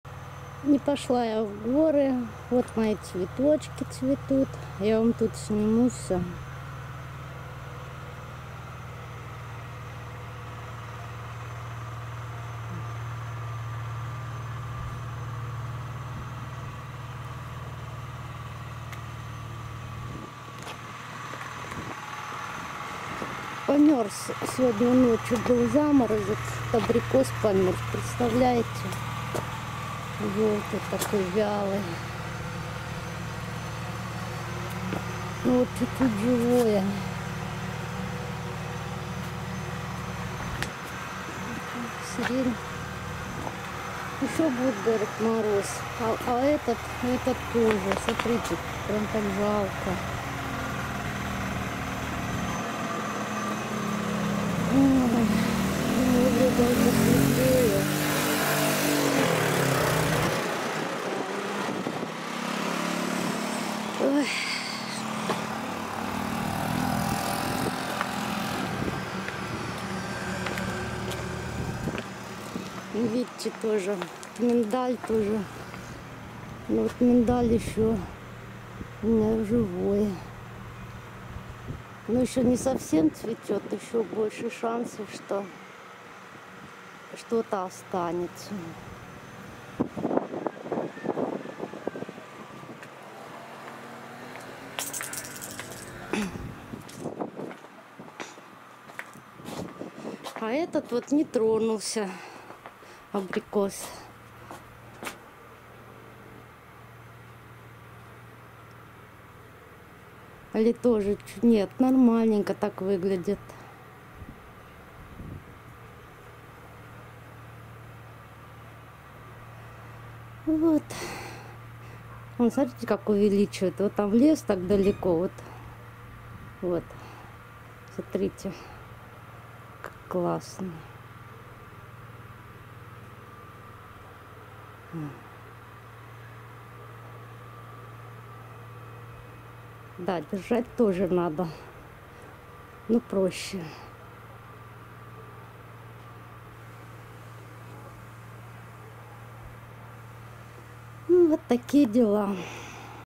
Был ночью заморозок и абрикос помёрз(((
не пошла я в сад, мимо проезжала, а он тоже помёрз((( Вот сняла у себя во дворе. У некоторых не помёрз, где чуточку защищено было зданиями.